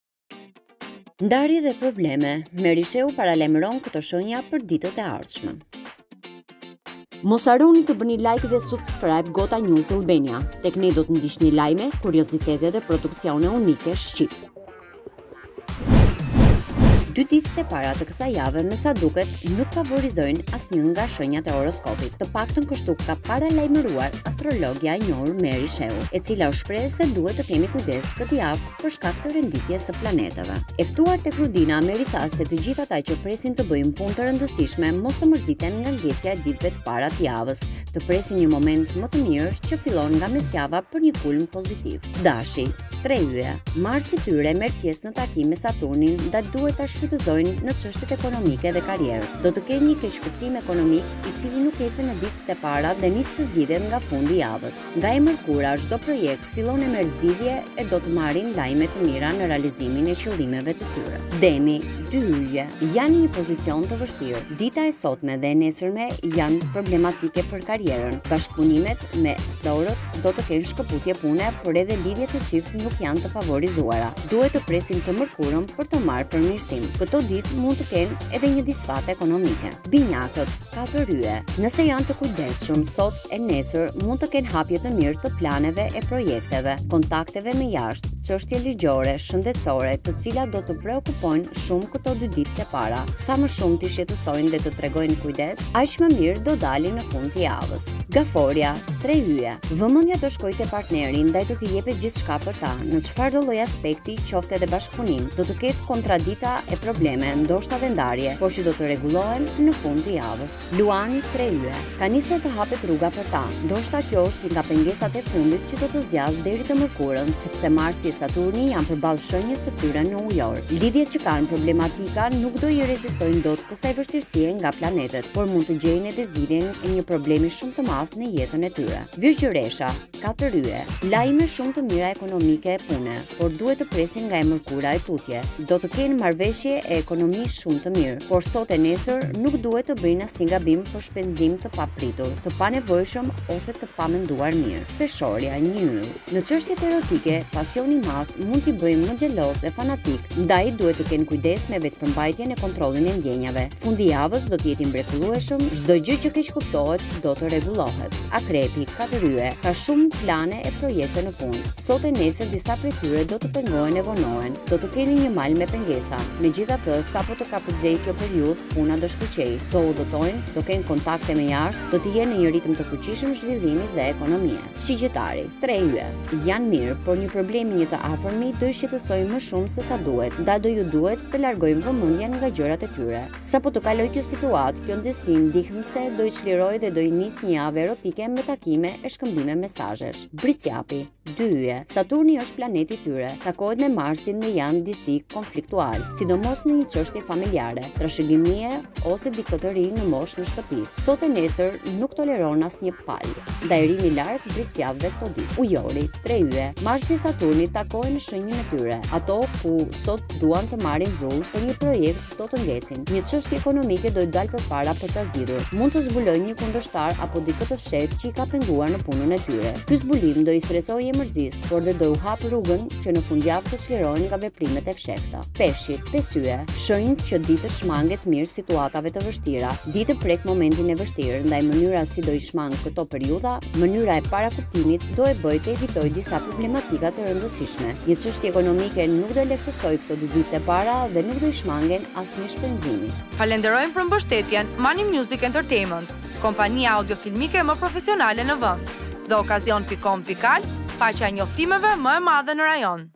Ndarje dhe probleme/ Meri Shehu paralajmëron këto shenja për ditët e ardhshme - Gota News
Ndarje dhe probleme/ Meri Shehu paralajmëron këto shenja për ditët e ardhshme

Follow planetapp.ltd

Follow silvifortstore